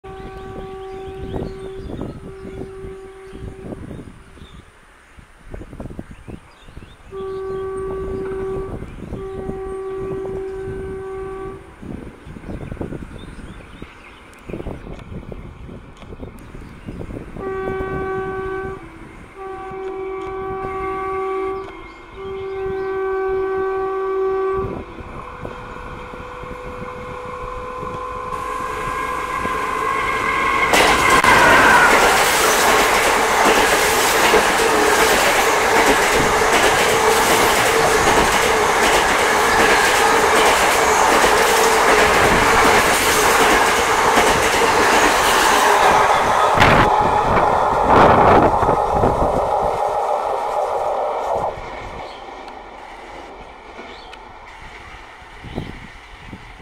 Dangerously Crossing || 12023 HWH~PNBE JAN SHATABDI EXPRESS AT FULL SPEED !!
Train number 12023 HOWRAH PATNA JANSHATABDI is a JANSHATABDI that runs between Howrah Jn and Patna Jn. It covers total journey of 532 kilometer and reaches its destination on day 1. It departs from Howrah Jn at 14:05 and arrives Patna Jn at 22:45 on first day. 12023 HWH PNBE JANSHATABDI has total 14 stopping stations in its route including both source and destination stations.

Device : Nikon Coolpix B500